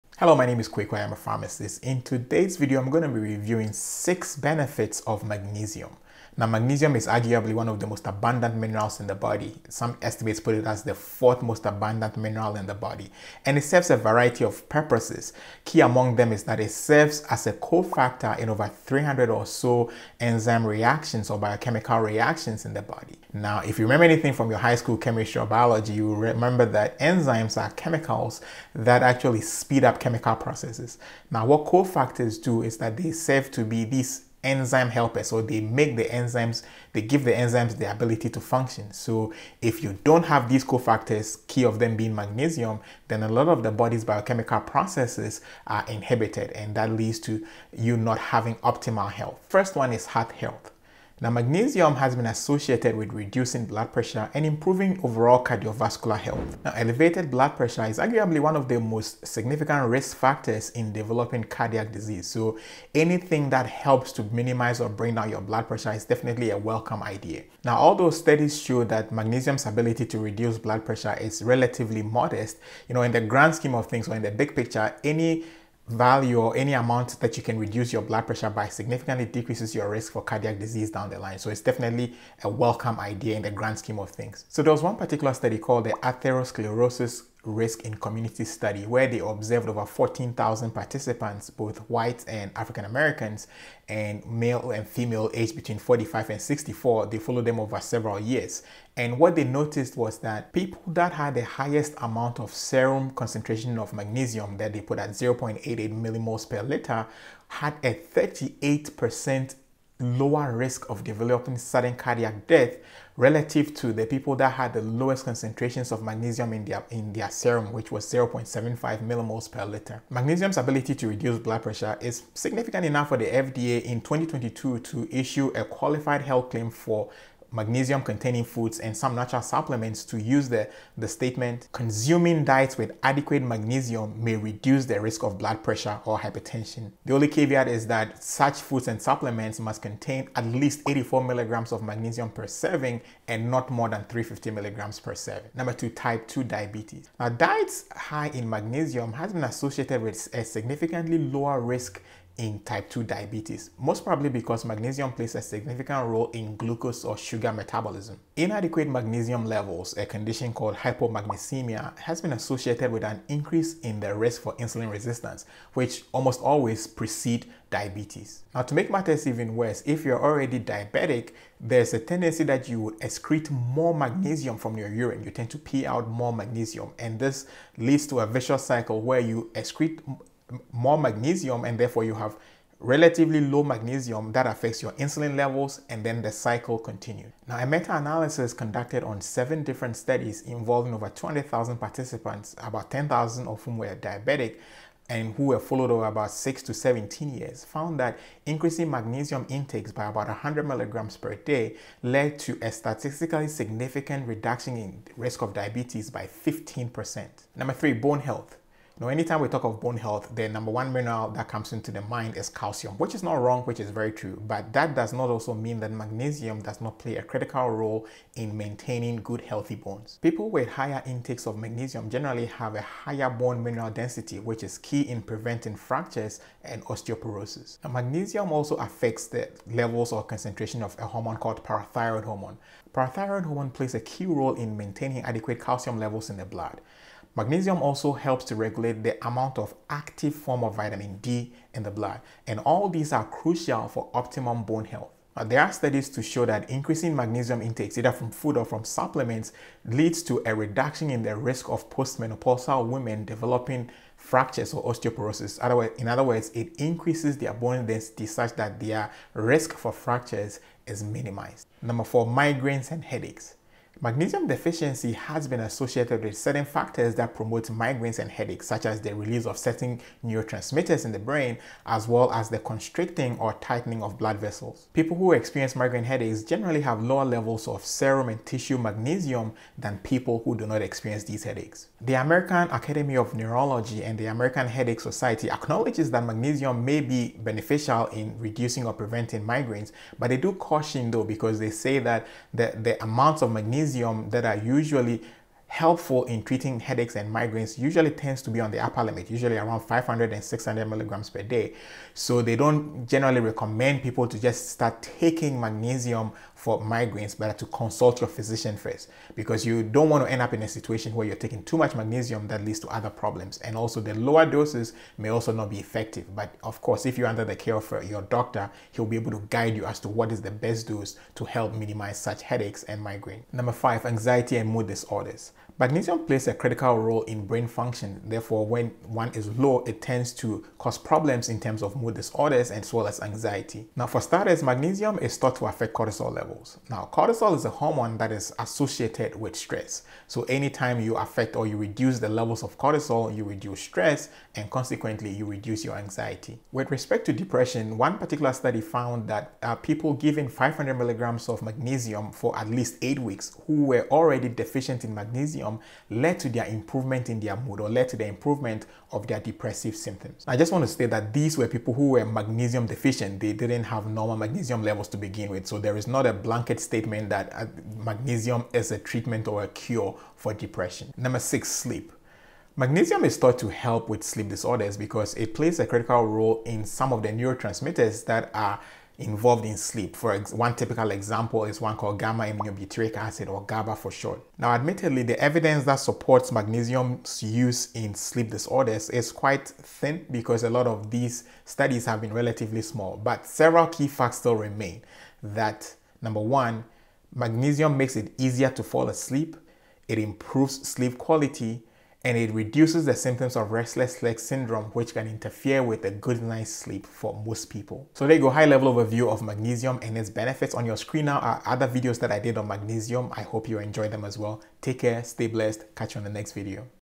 7 Amazing Benefits Of Magnesium | Why You Need Magnesium
Magnesium is important for many processes in the body, including regulating muscle and nerve function, blood sugar levels, and blood pressure and making protein, bone, and DNA.
In this video I discuss 6 key benefits that may be derived from having adequate magnesium levels.